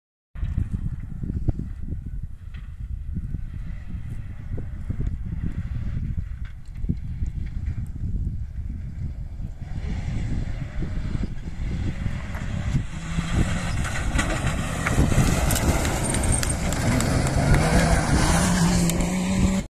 Rally Baja Aragón 2024 X-RAID MINI JCW TEAM PRT Joao FERREIRA PRT Filipe PALMEIRO
Voy poniendo videos de mis animales y de las cosas que hago.